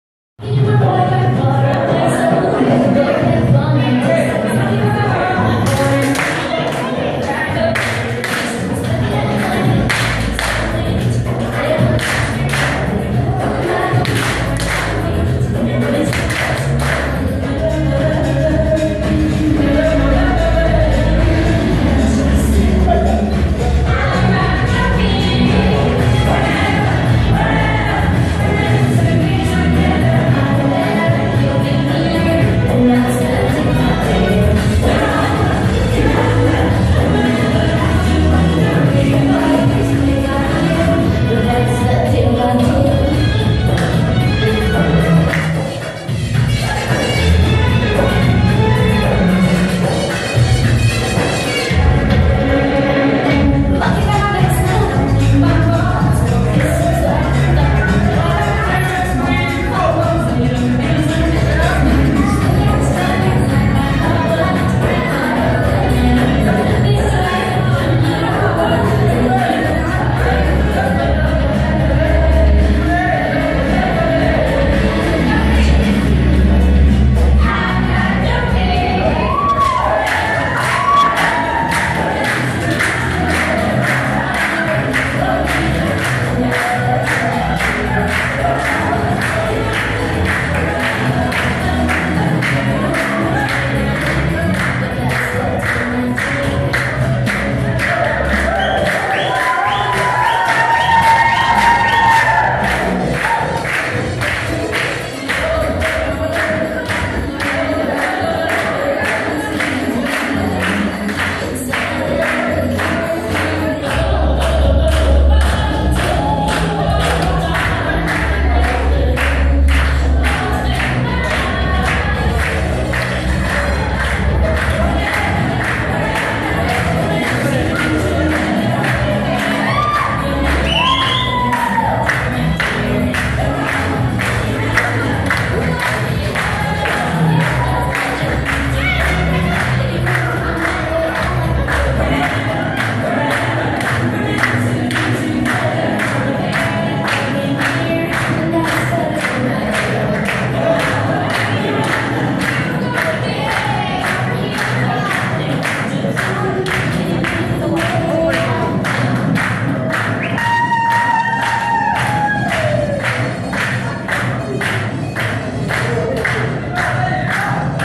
Lioué Dance Zumba® Belly dance shakira
Une sperbe ambiance avec des gens que j'aime à Viking Bardo :)